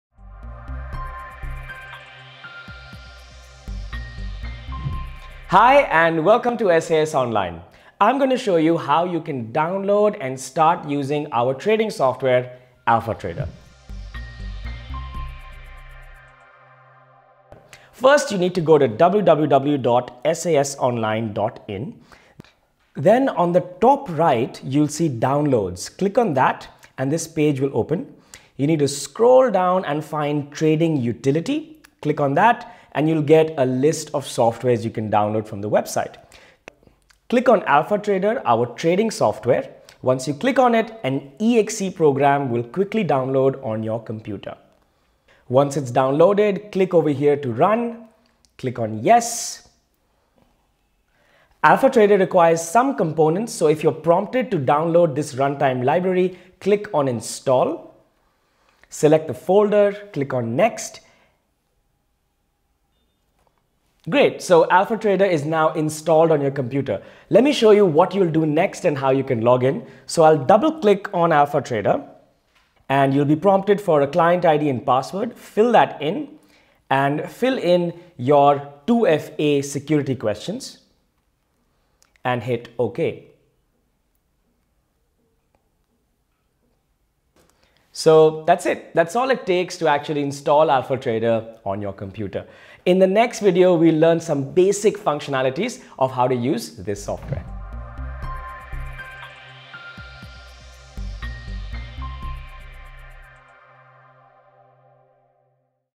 Alpha Trader Download Install Login (Desktop)
Here is the details about Alpha Trader, how to download and install Alpha Trader for Desktop users.